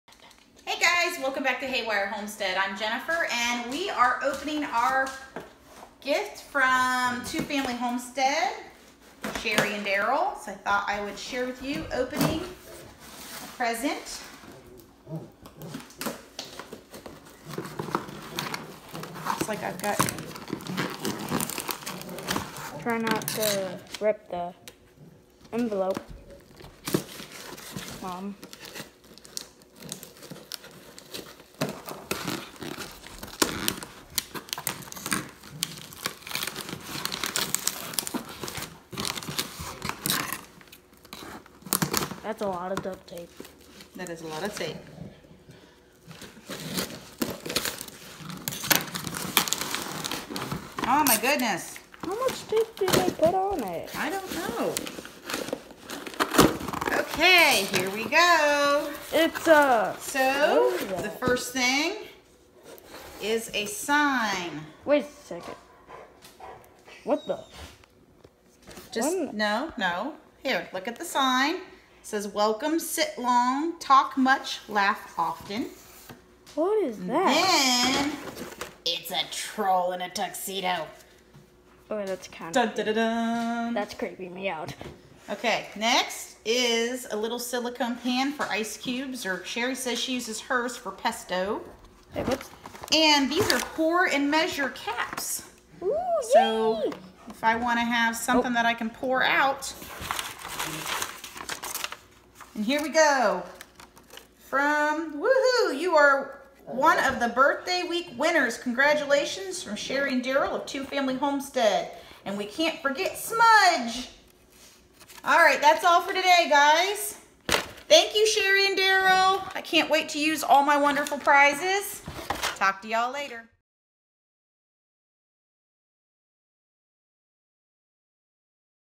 I never win anything!
I never win anything! So it was a huge surprise when I won one of the gifts for Sherri of Two Family Homesteads birthday week giveaway.
Thank you Sherri the troll may be my fav part!!!